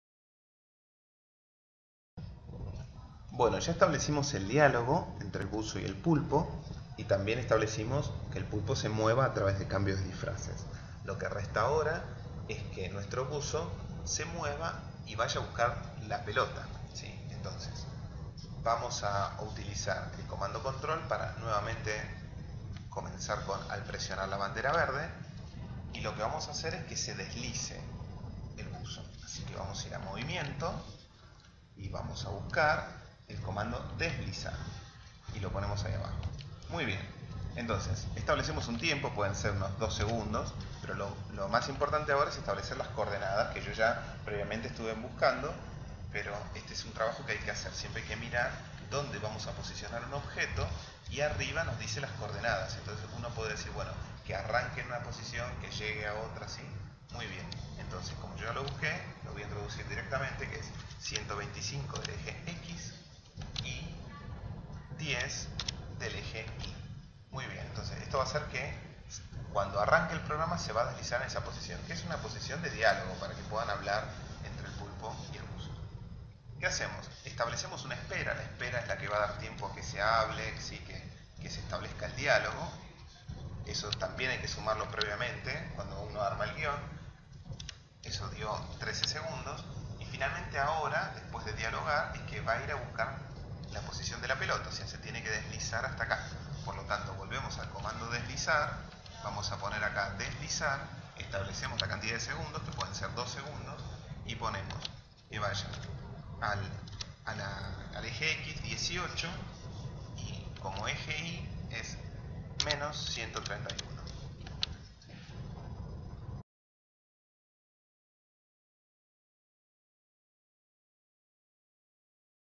Programación con Scratch: 6 - Buzo. Desplazamientos
Video tutorial que explica cómo desplazar los objetos a una determinada posición, con Scratch. Perteneciente a la Jornada EMI de octubre de 2014.